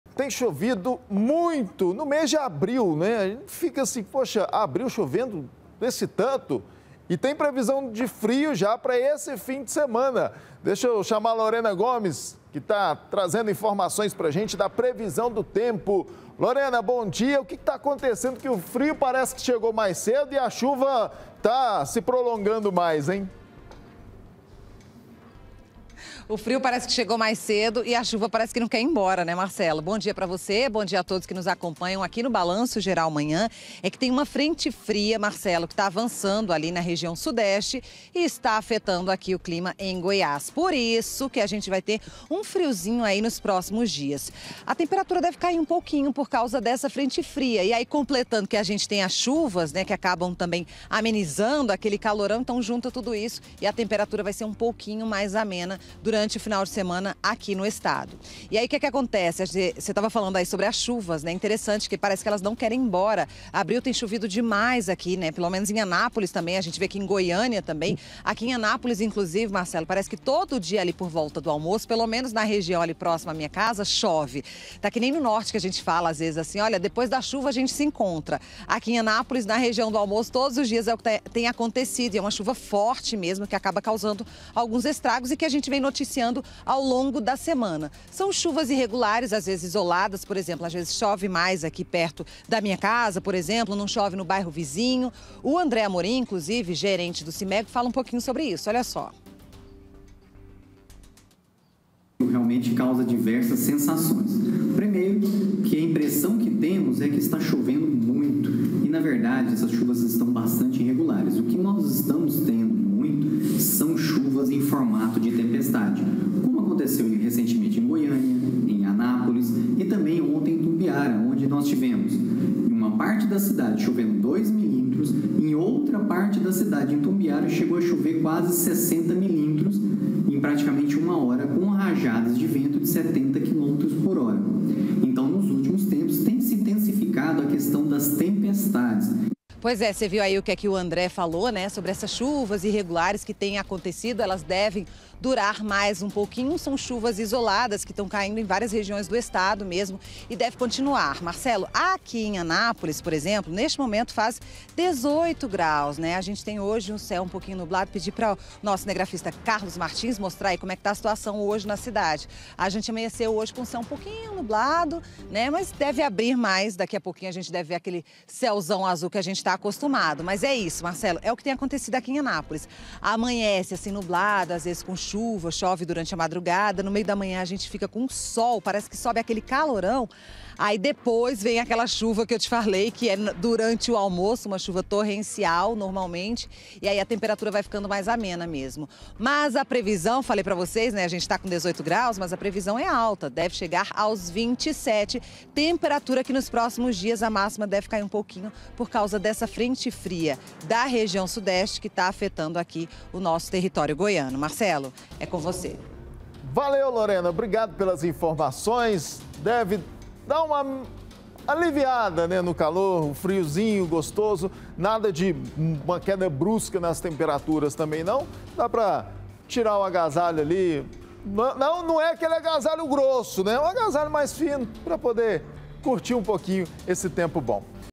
FRIO PRECOCE: FRENTE FRIA CHEGA A GOIÁS NO FIM DE SEMANA
TEM CHOVIDO DEMAIS EM ABRIL E TEM PREVISÃO DE FRIO PRA ESSE FIM DE SEMANA